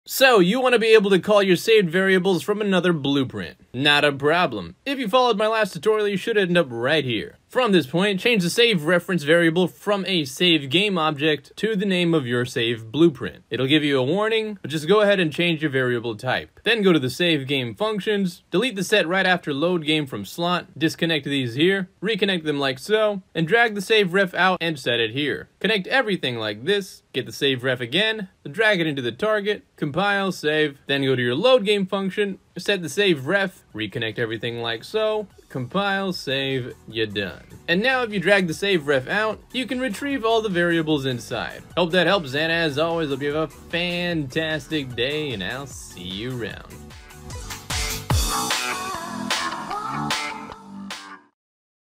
Unreal5 Blueprints: Referencing SAVED Variables - (60-SECONDS!!)
Here's one way you can reference Saved variables from another blueprint in Unreal5-!!